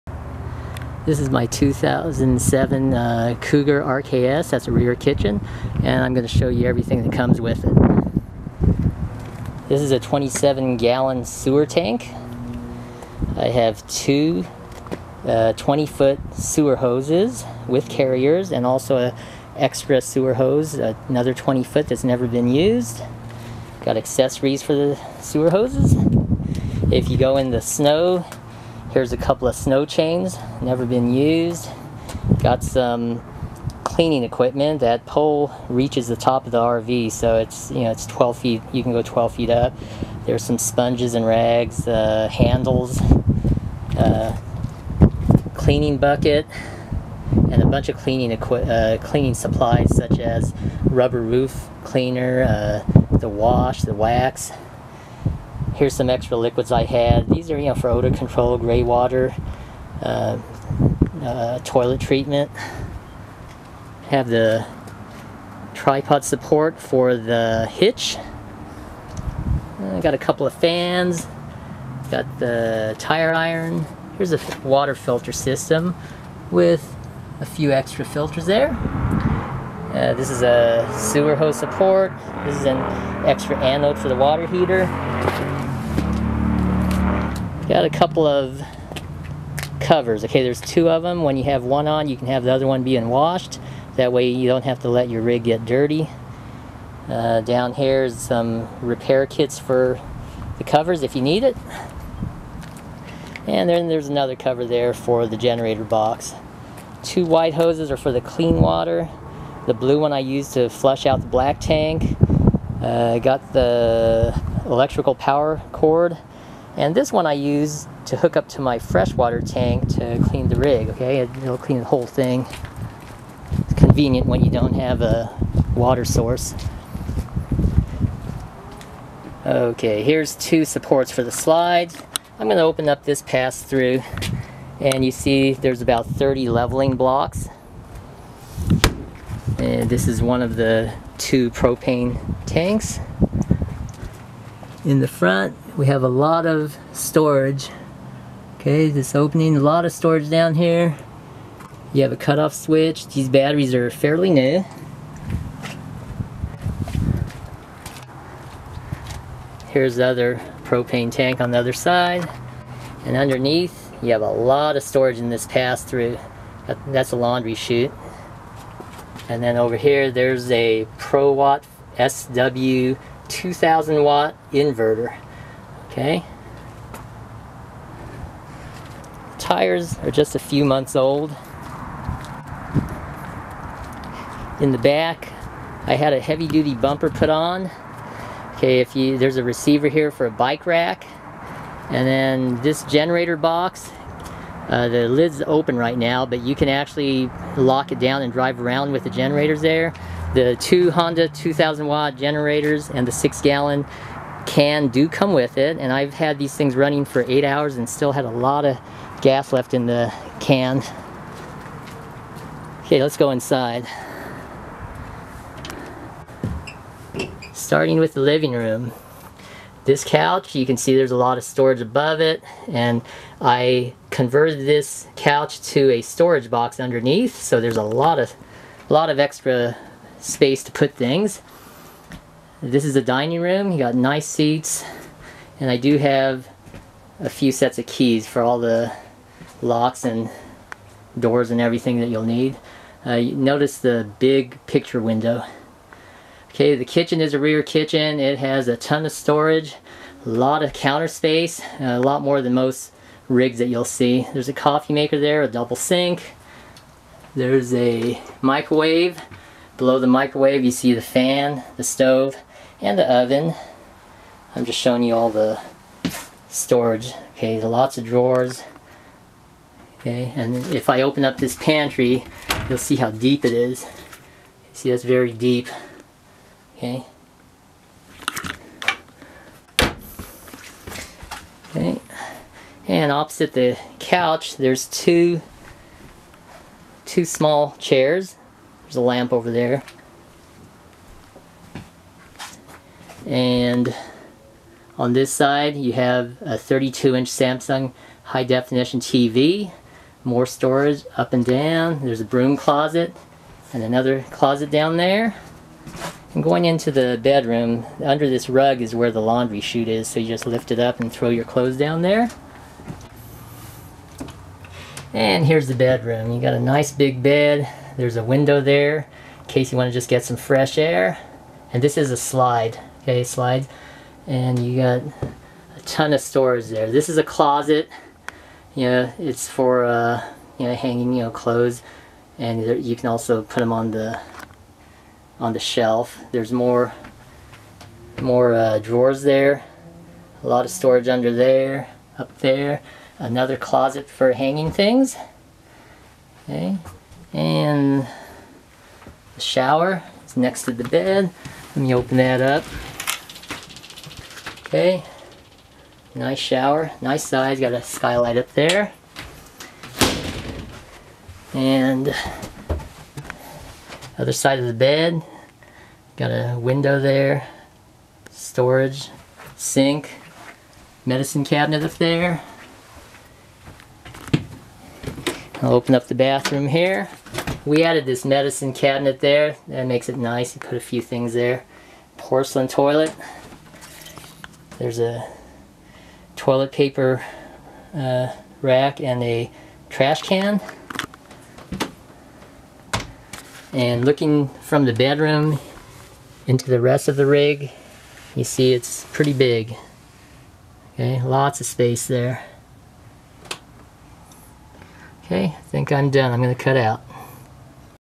2007 KEYSTONE 292rks 25500 in Stevenson Ranch, CA
2007 KEYSTONE 292rks 25500 in Stevenson Ranch, CA for sale on RVTrader.com